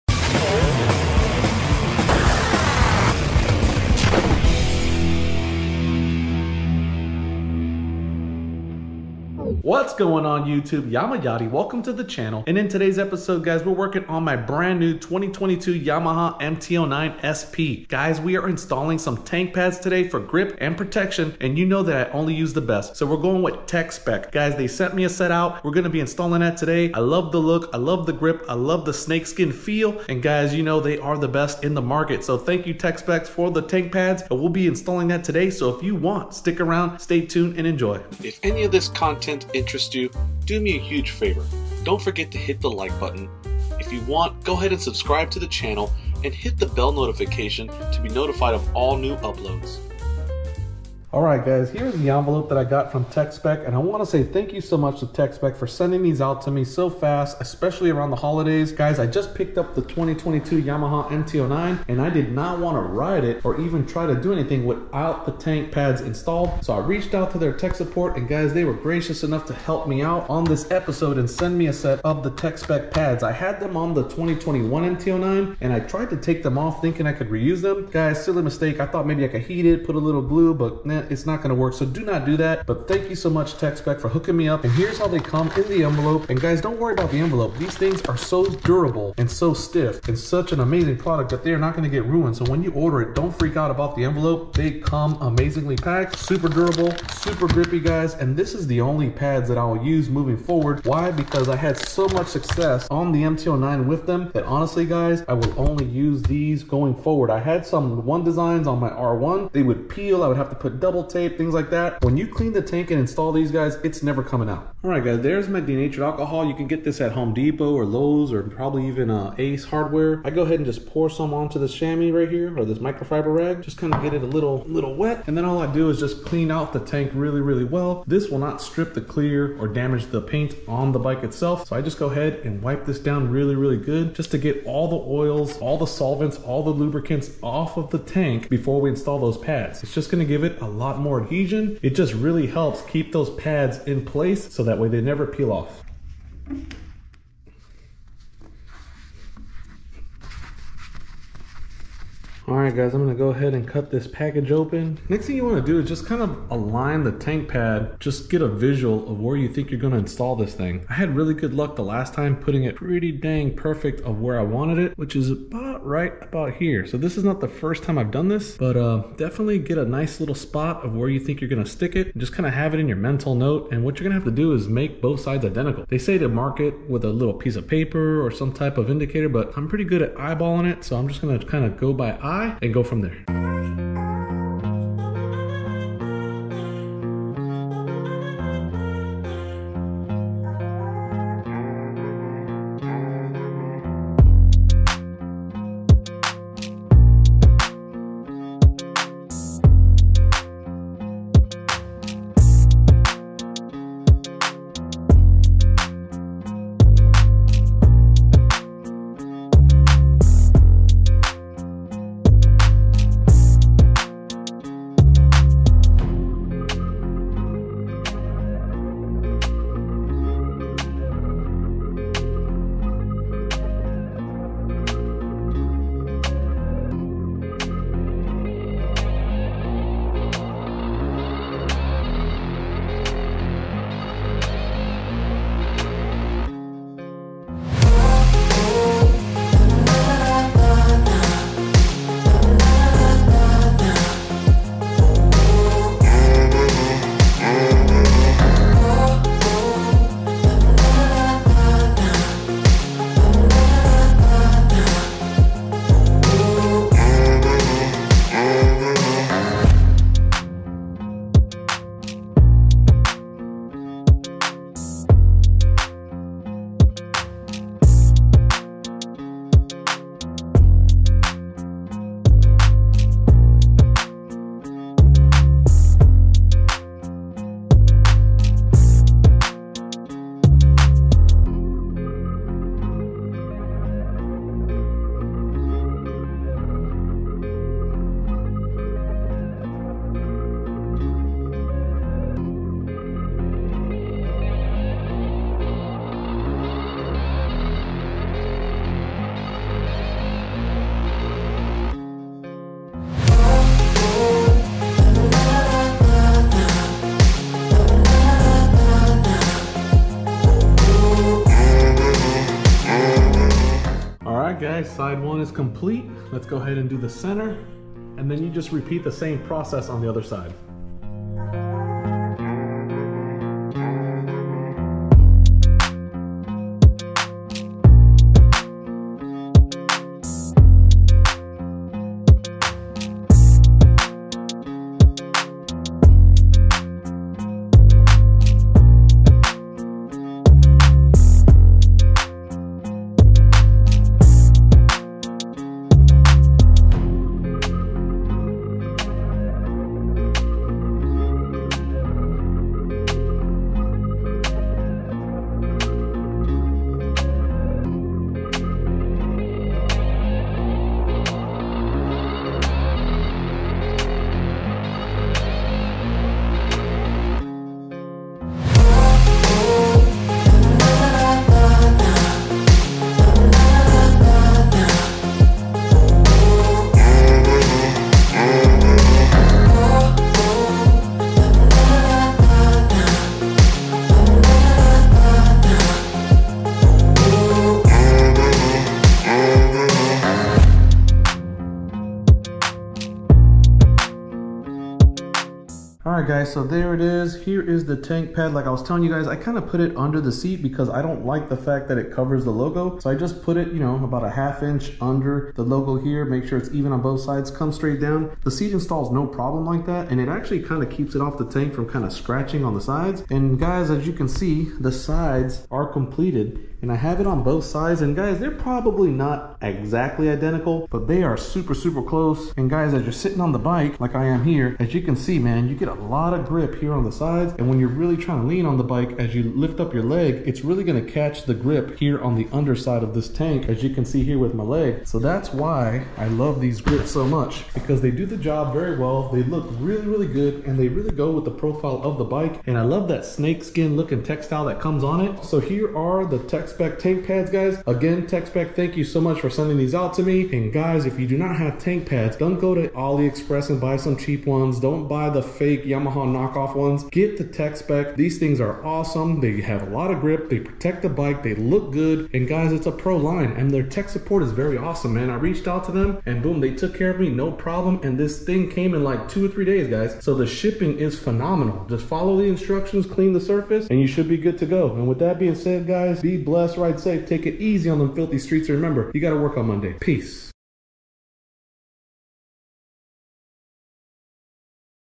2022 Yamaha MT09 SP Best Tank Grip & Protection (TechSpec) install.
In this video we install TechSpec tank grips on my 2022 Yamaha MT09 SP.